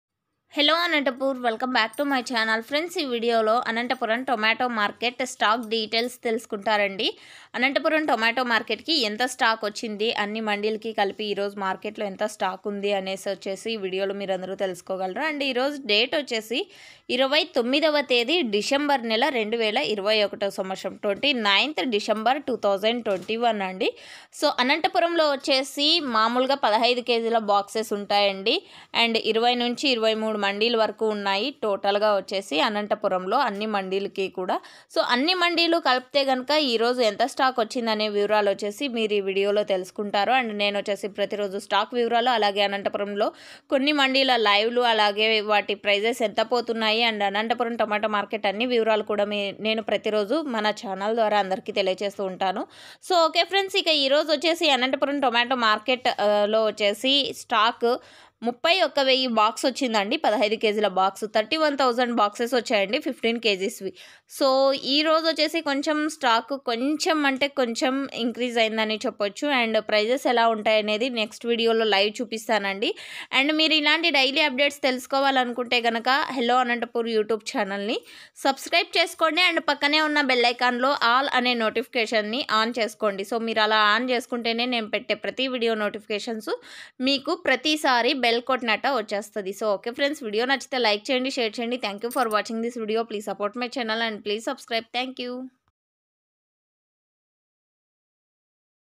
28-12-2021 today Anantapur tomato stock/today tomato price/Anantapur tomato rate
2022 జనవరిలో టమోటా ధరలు/next month tomato rates/రాబోయే రోజులలో టమోటా ధరలు @Hello Anantapur